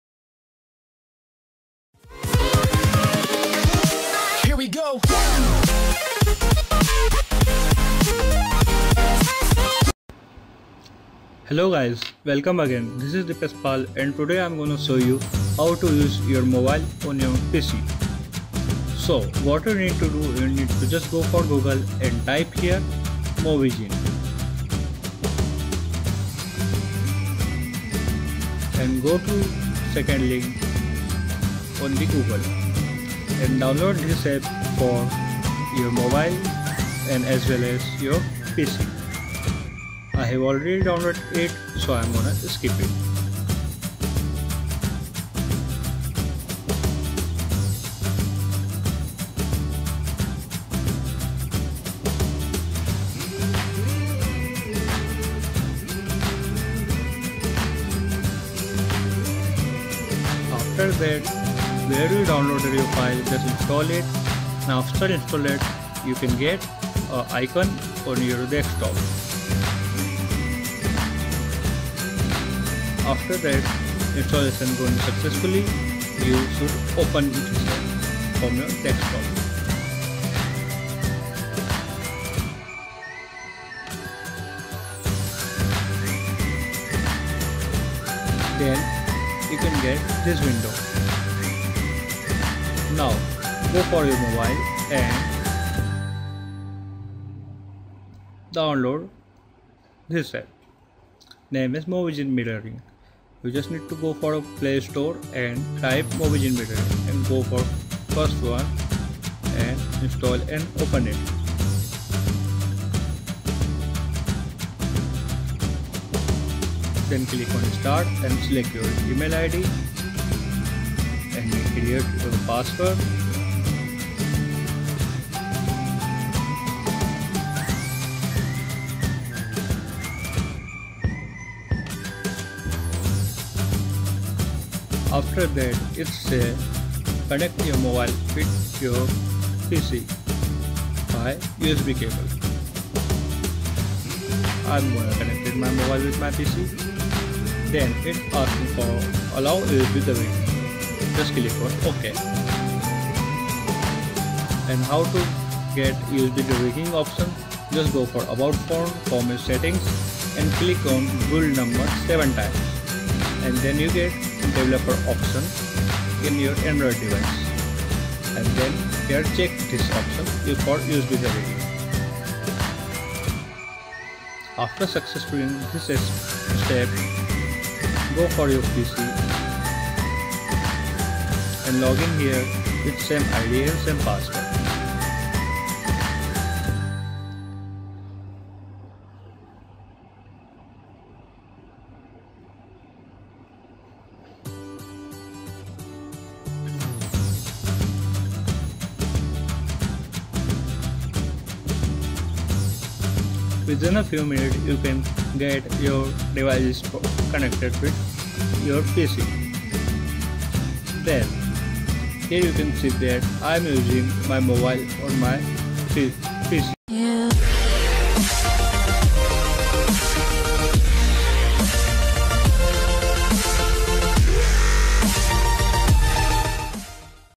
How to use mobile in pc | Cast your mobile in pc
In this video I will show you How to use mobile in pc or mirror your android in pc.
Internet connection require.
LINK -